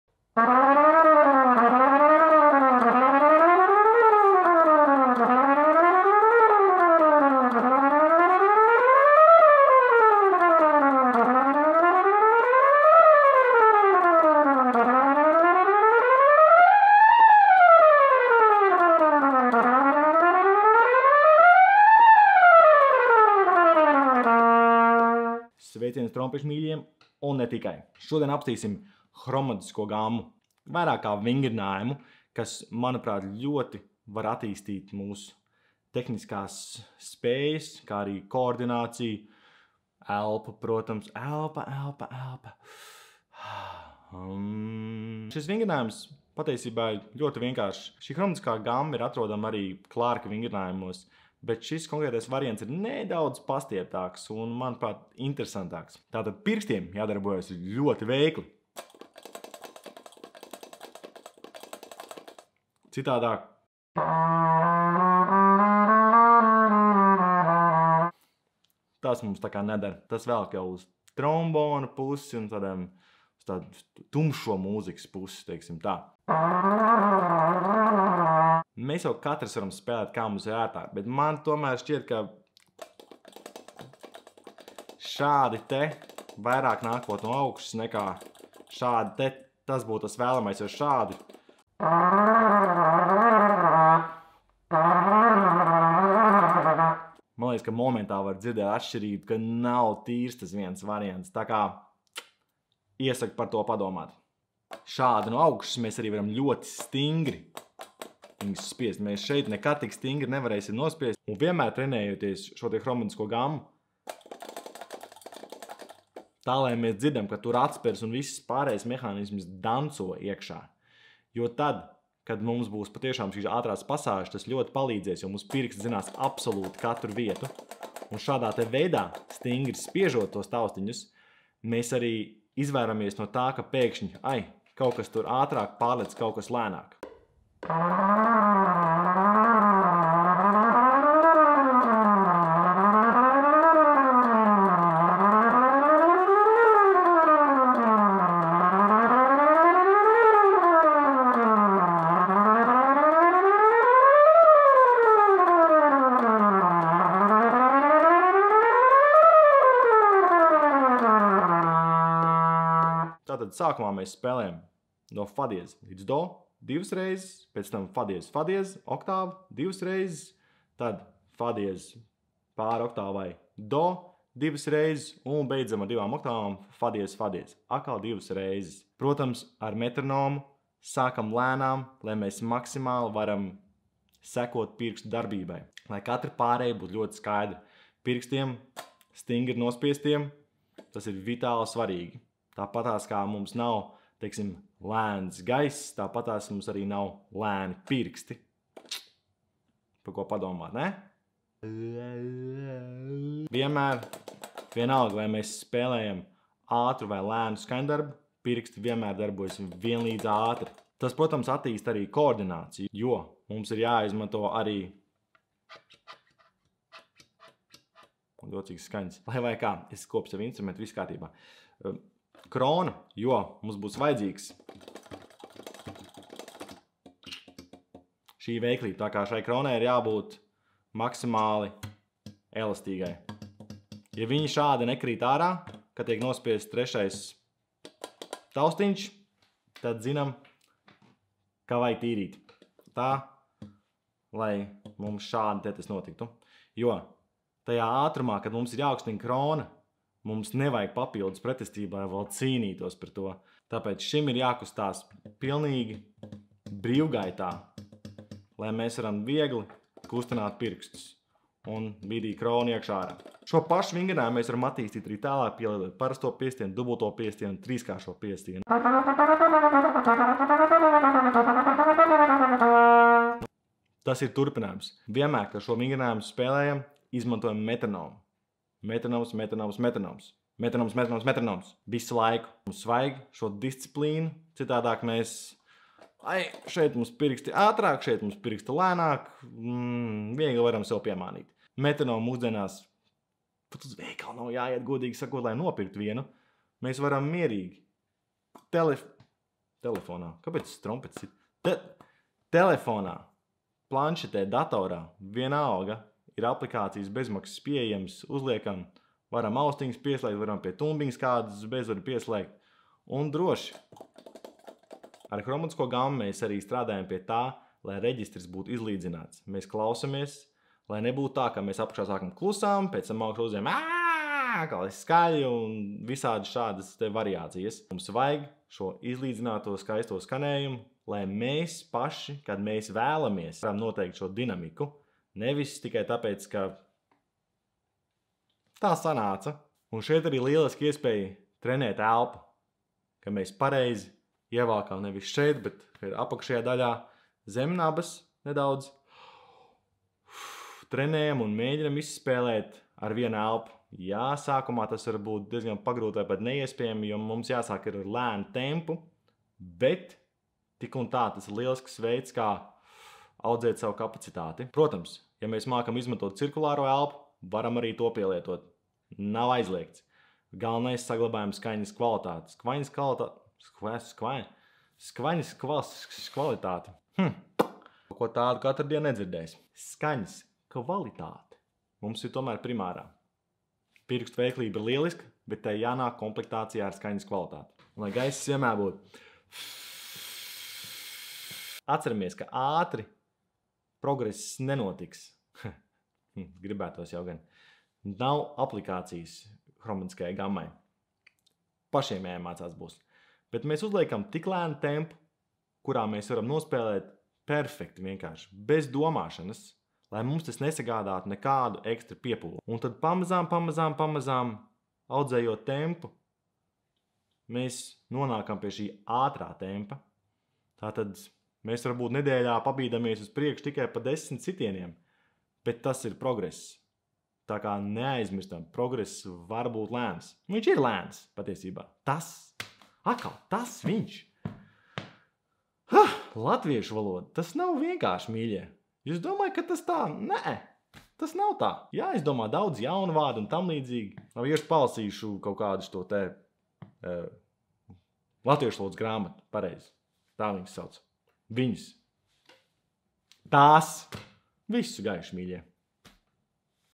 Par un ap hromatisko gammu
Kā paplašināt hromatisko gammu spēlēšanu un kas būtu jāievēro to darot + vēl pāris interesantas lietas! Lai izdodas!!!